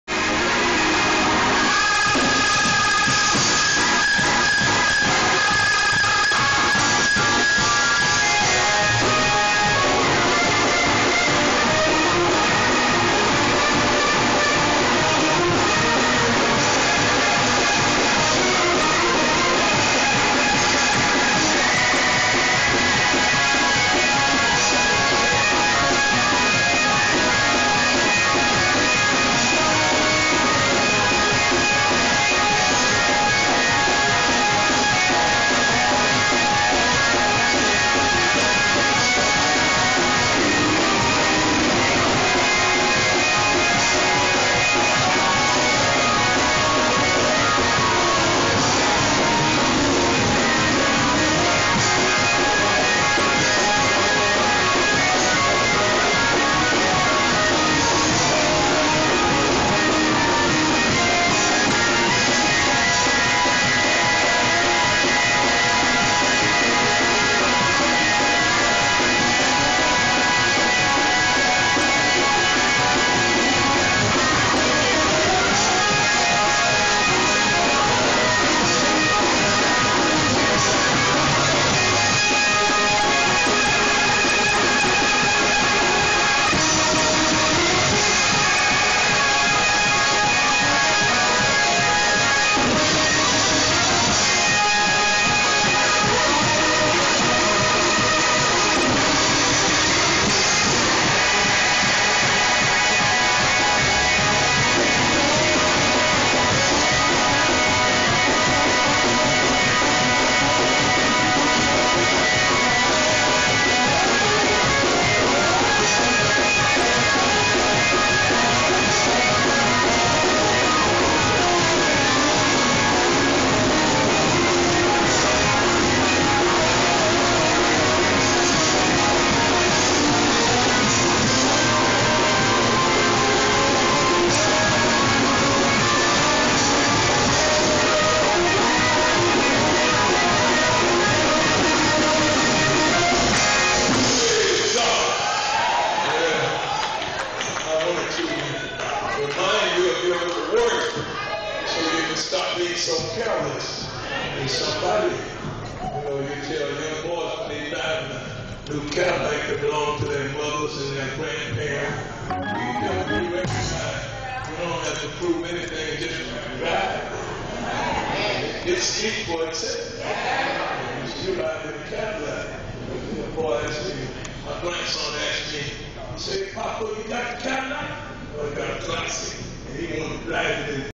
SW MI #1 66th Annual Holy Convocation Opening Address Sermon Bishop John Henry Sheard Part 3
SW Michigan Jurisdiction #1 66th Annual Holy Convocation Opening Address August 21, 2012 12:00 Noon Bishop John Henry Sheard, Prelate and Chairman of the Board of Bishops of the Church of God In Christ

Held at Bailey Cathedral COGIC - Southwest Michigan Jurisdiction COGIC #1 State Headquarters

Elder Robert Moore - Hammond Organ
Tony Farrier - Piano/Keyboard
Terrence Curry - Trombone
Marcus Billings - Drums